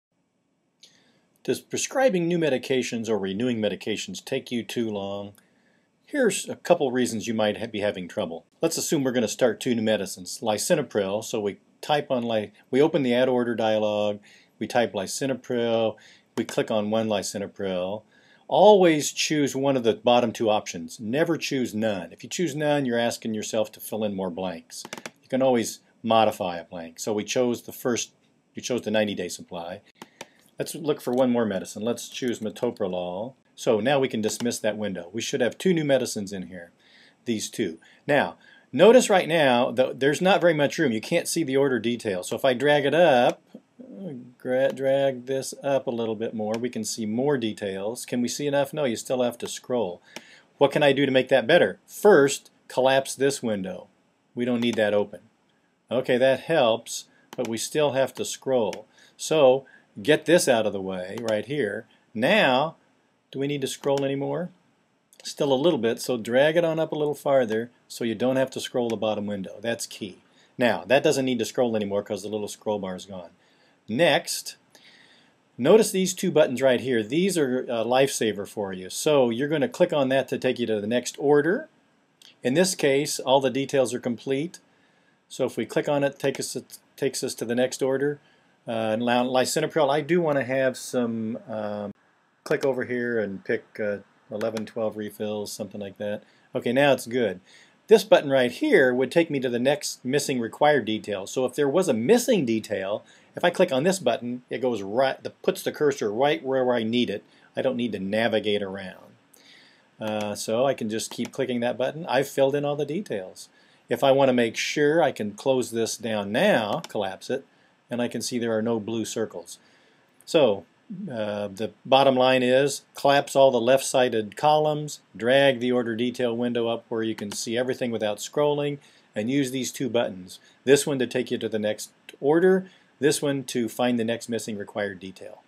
Prescribing Shortcuts in Cerner PowerChart
Resizing windows to avoid scrolling is key. There are two little buttons you might not have noticed before. They want to be your friends.